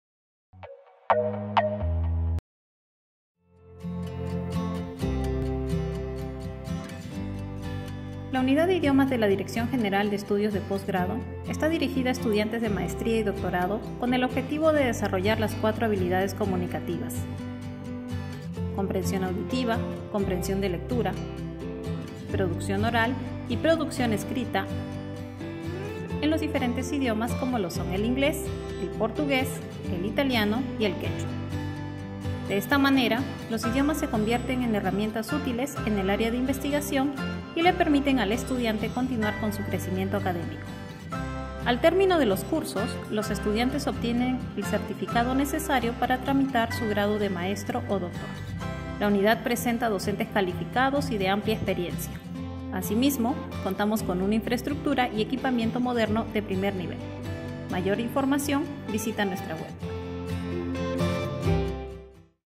Programa de Idiomas Extranjeros - Posgrado UNMSM 2022
¿Estudias un Posgrado en la Universidad Nacional Mayor de San Marcos?

Ponemos a tu disposición el Programa de Idiomas de Posgrado, con el fin de prepararte para rendir el examen de suficiencia y así obtener el grado académico de Magíster o Doctor.